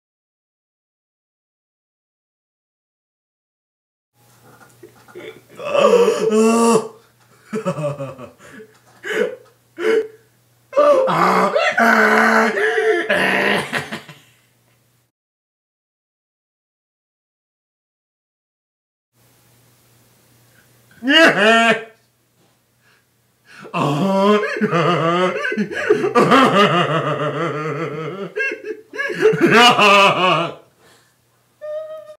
Charlie and Bob as donkeys.....comment plz
k, so my dad and i are acting like donkeys, hope you all like it and laugh really hard